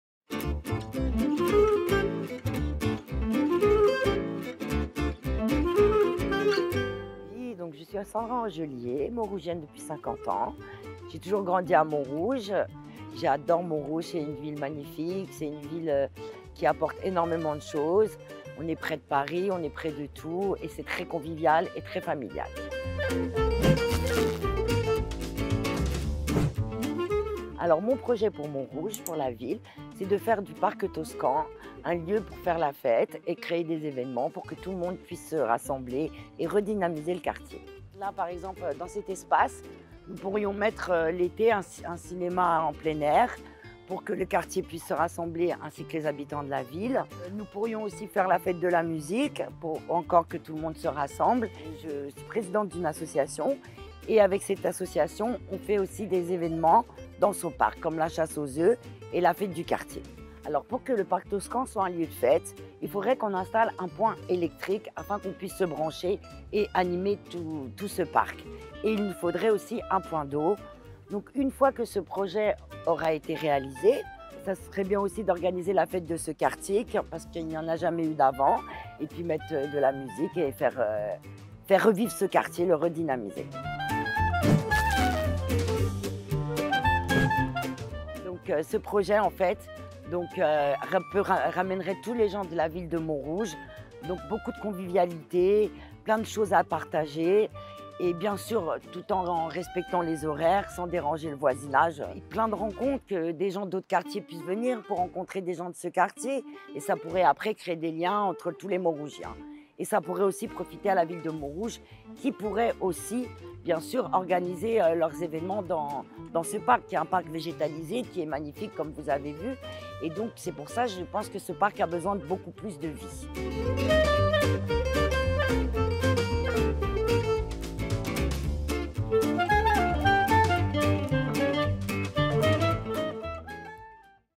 Le projet de Sandra ANGELIER - Budgets Participatifs Montrougiens
Créer un lieu sympathique pour tous les Montrougiens pour leur permettre de profiter d'animations de qualité dans leur Ville comme par exemple : une scène musicale ou des pique nique avec des animations, des fêtes de quartier, fête de la musique etc.